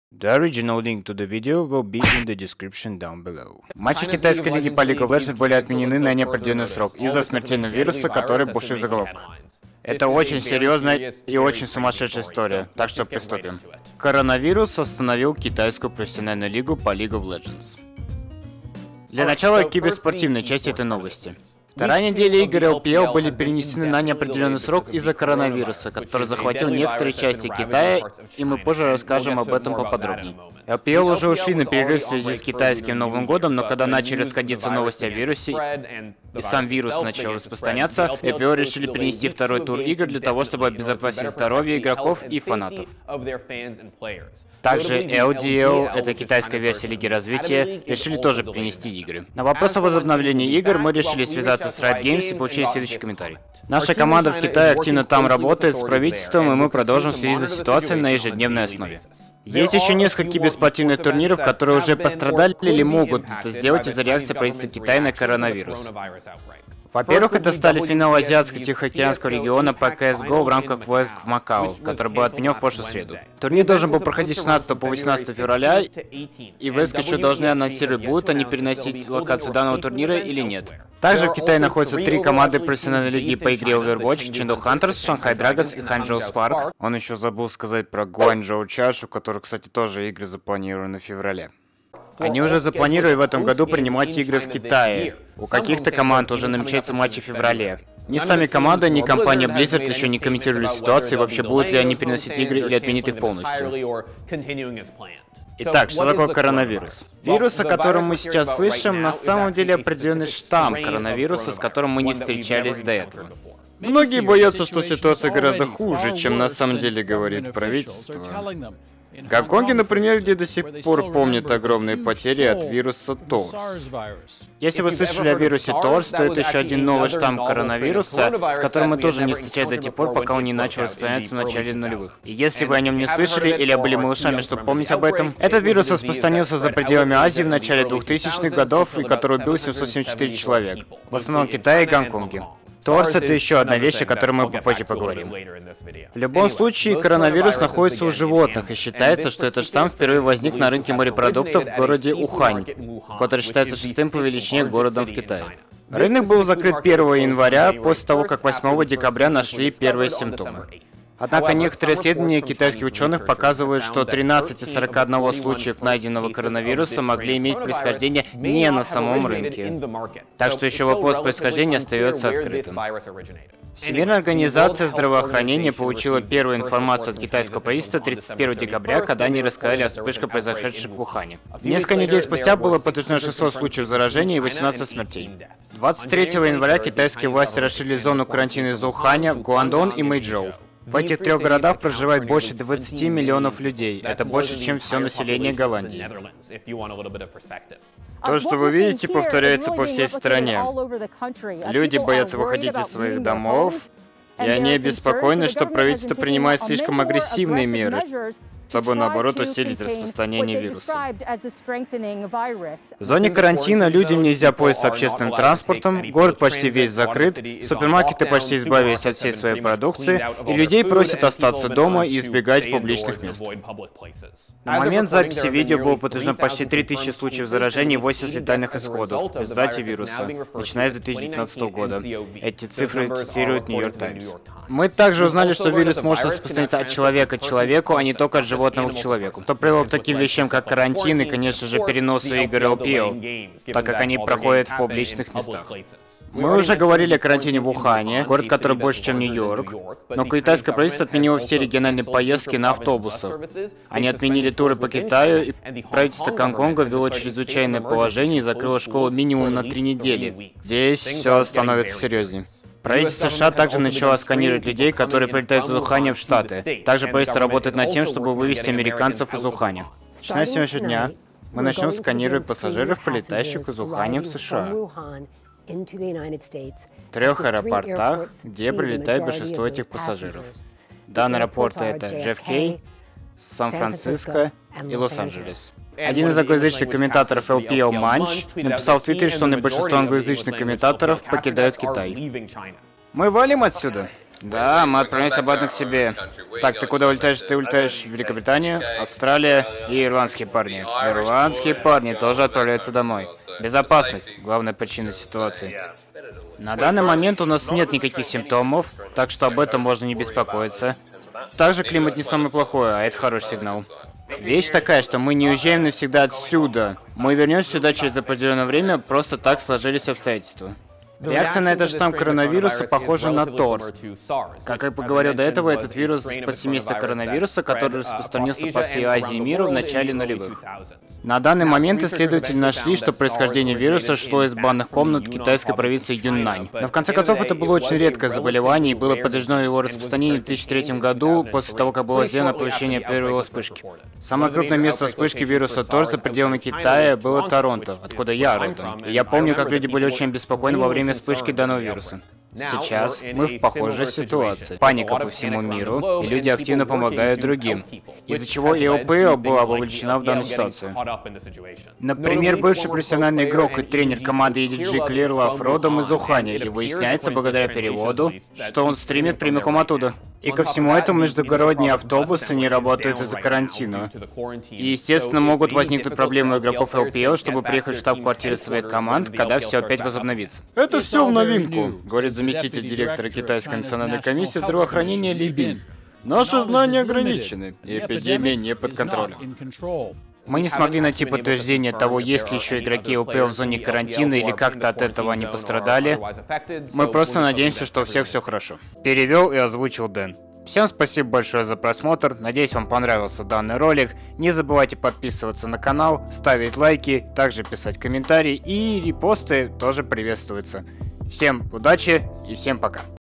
КОРОНАВИРУС ОСТАНОВИЛ КИБЕРСПОРТ! | RUS Перевод
Недавно в Китае началась массовая вспышка Коронавируса. Данное событие повлияло не только на жителей провинции, но и на киберспортсменов.
В этом видео о том, как Профессиональная лига в Китае остановила матчи по League Of Legends и когда всё это возобновиться.